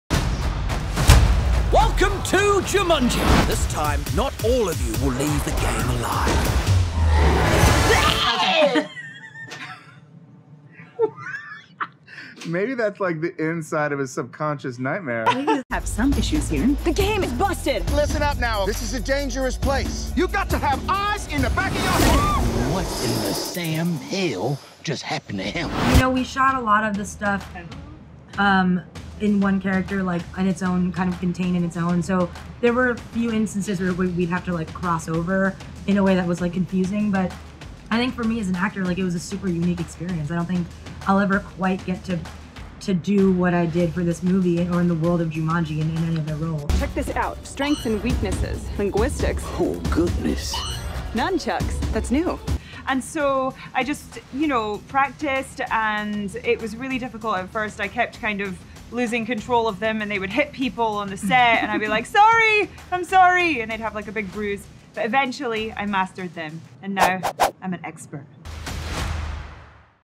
Close Up: Awkafina, Jack Black and Karen Gillan
Awkafina, Jack Black and Karen Gillan laugh it up in Landmark's sit down to discuss JUMANJI: THE NEXT LEVEL